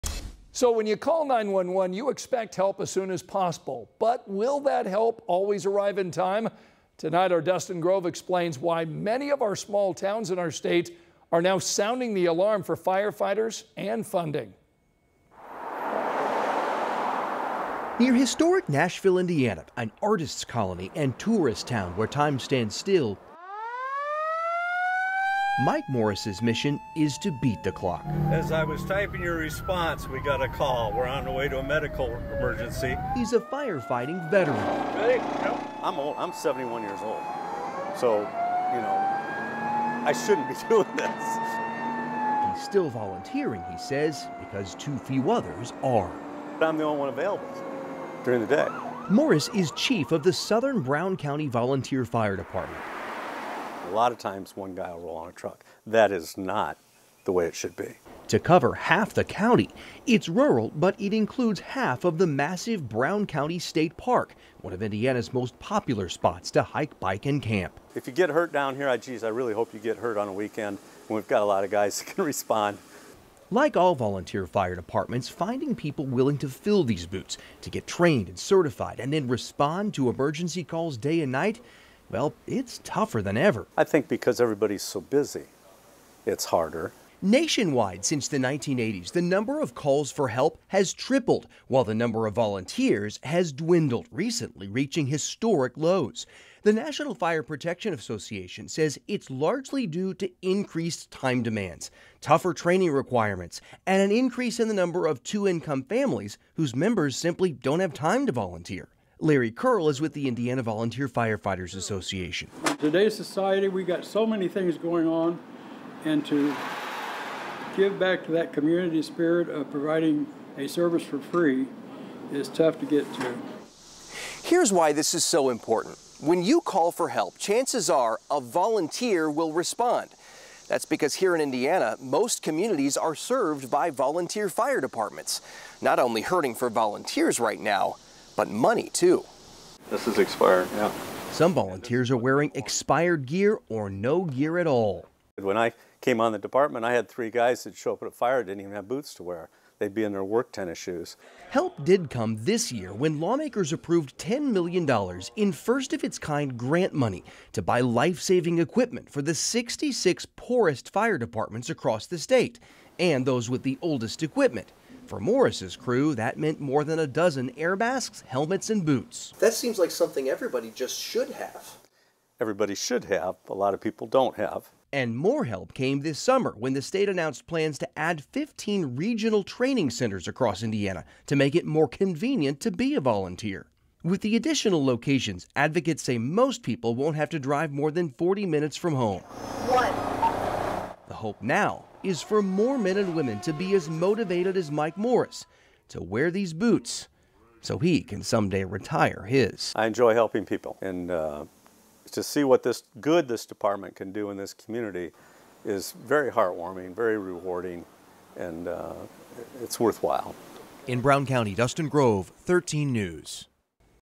Fight for firefighters
Many communities in Indiana are sounding the alarm for support of first responders.